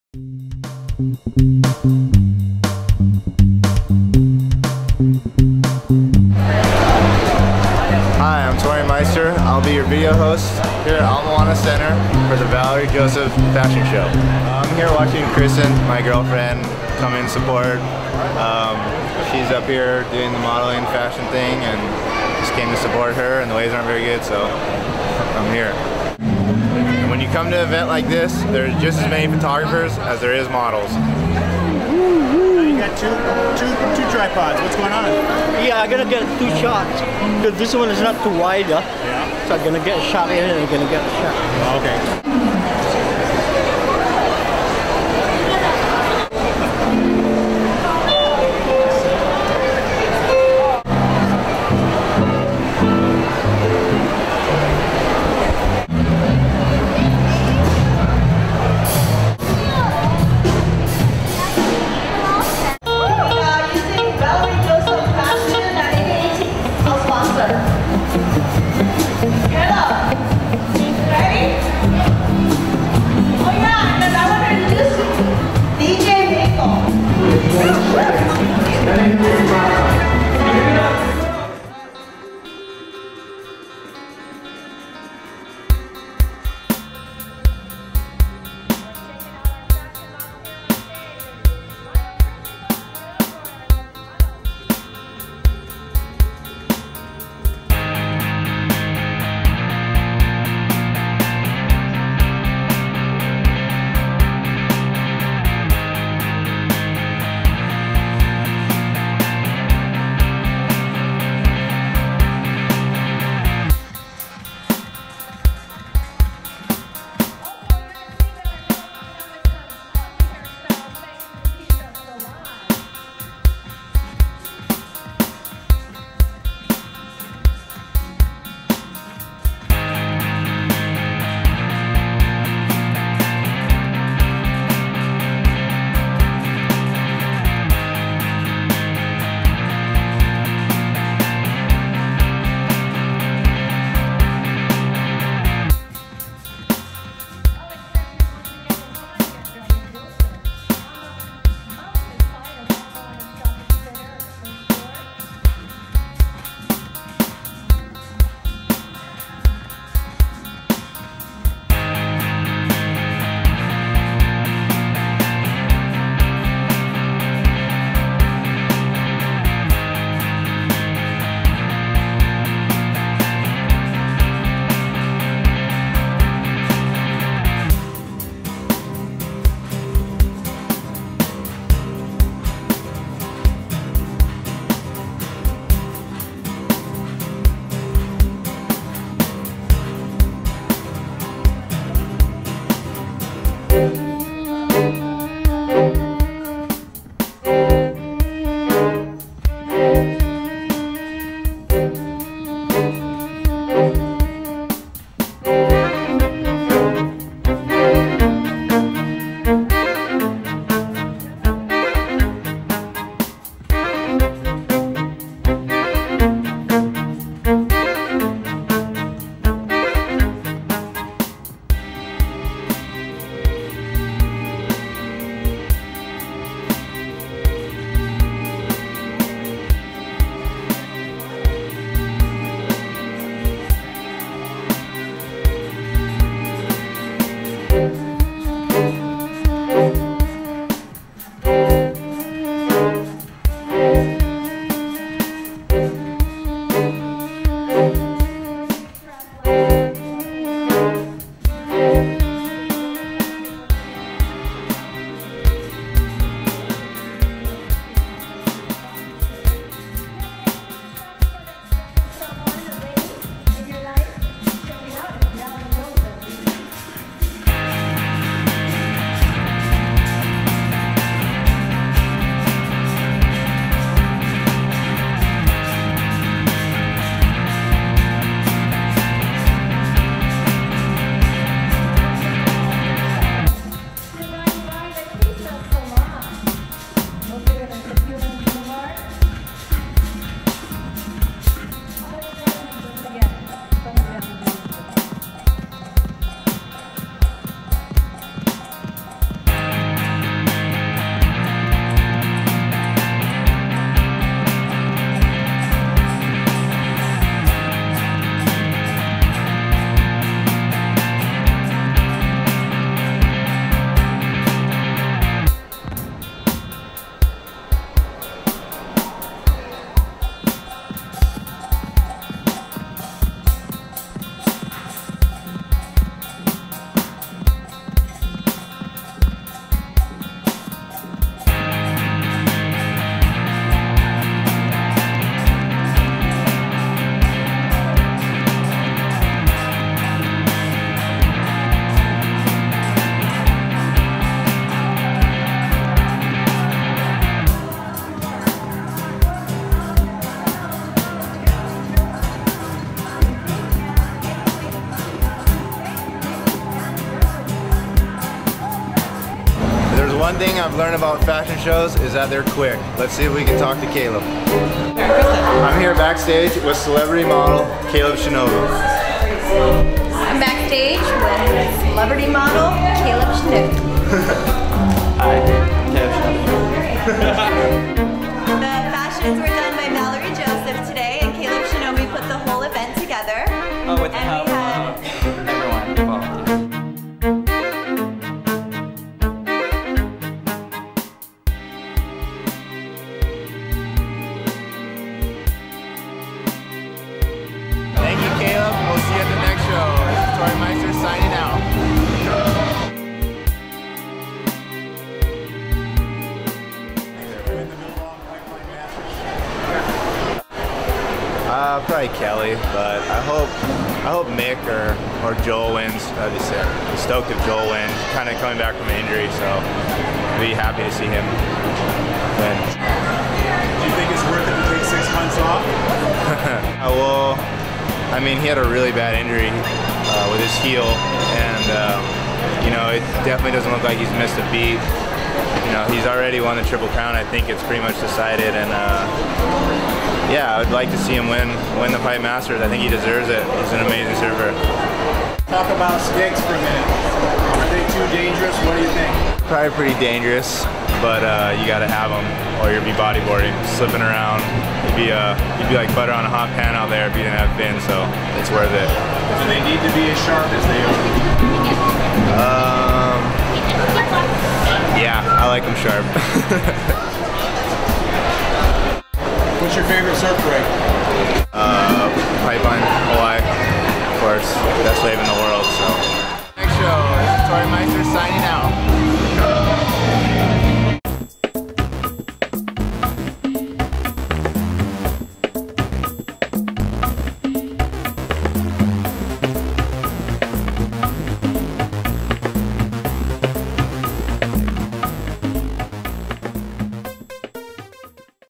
Valerie Joseph Fashion Show with Torrey Meister
Billabong Pro Surfer Torrey Meister goes to Ala Moana Center Stage to watch his fashion model girl friend Kristen participate in a fashion show. Torrey decides he will be the video host to practice his on camera skills.